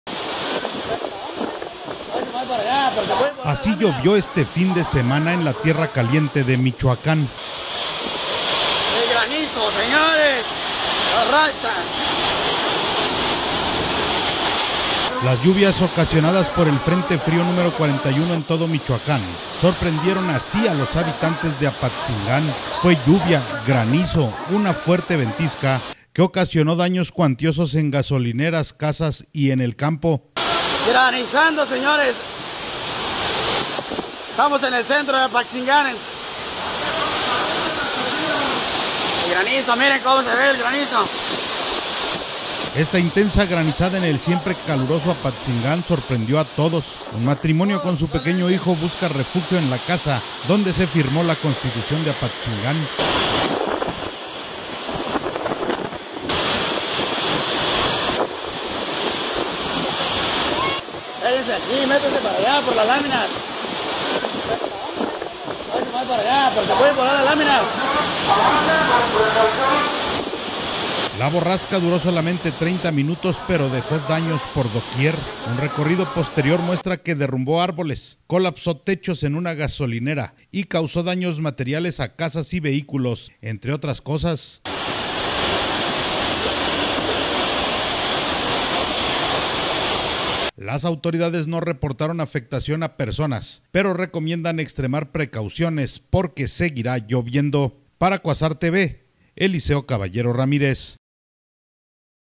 Feroz granizada en Apatzingán
Así llovió este fin de semana en la tierra caliente de Michoacán…